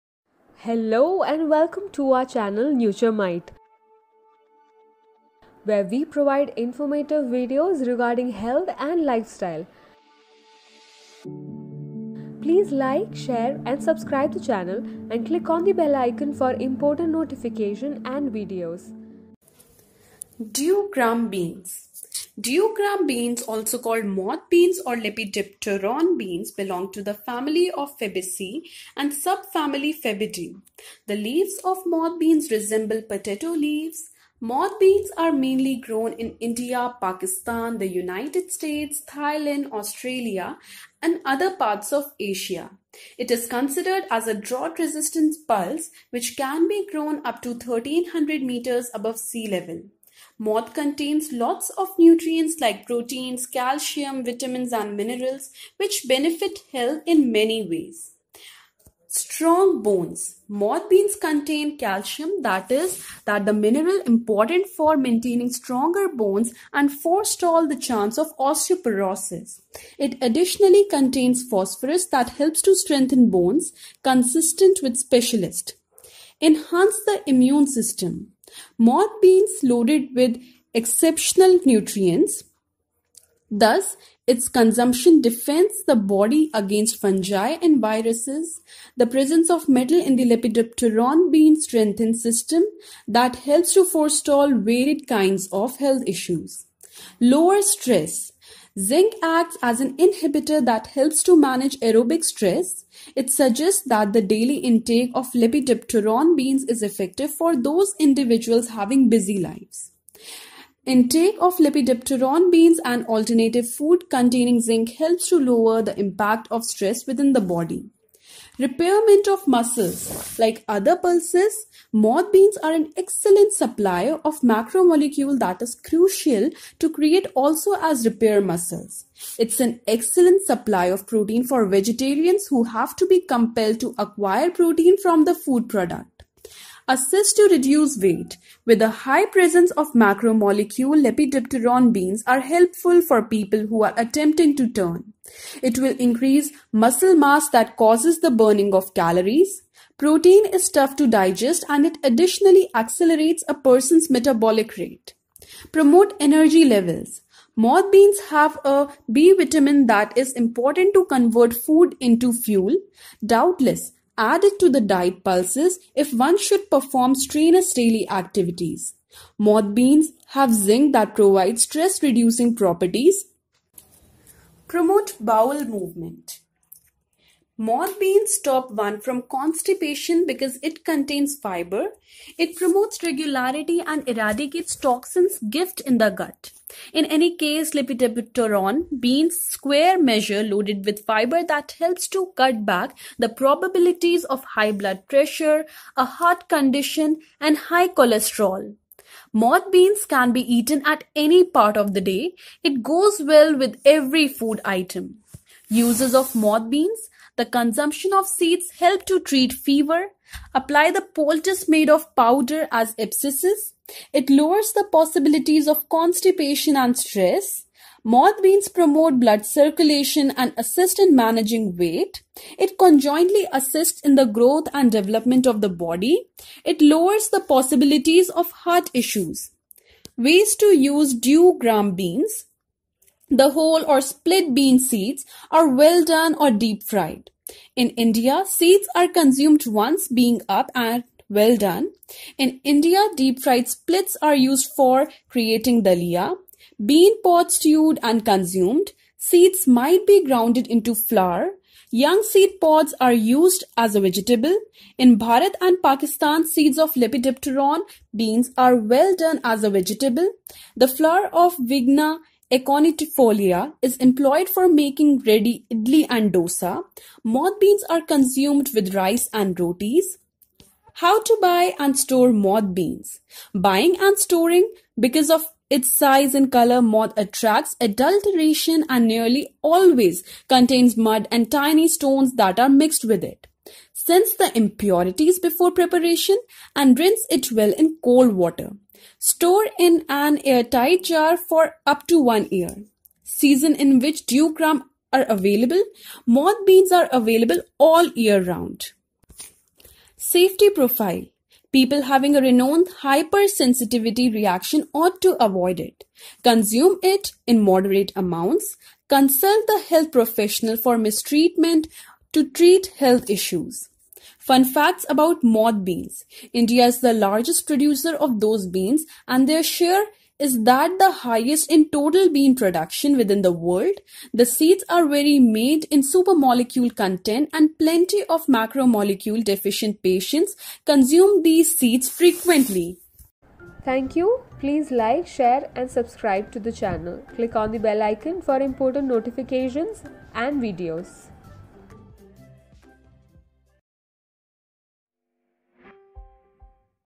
5 Health Benefits, Nutritional Values Dew Gram Beans/Sprout Bean / Moth Beans for Weight Loss
Dew gram beans also called moth beans or lepidopteron beans belong to the family of Fabaceae and sub-family faboideae. The leaves of moth beans resemble potato leaves.

Moth beans are mainly grown in India, Pakistan, the united states, Thailand, Australia, and other parts of Asia. it is considered a drought-resistant pulse that can be grown up to 1300 meters high.

Moth contains lots of nutrients like proteins, calcium, vitamins, and minerals which benefit health in many ways.

For More information watch the video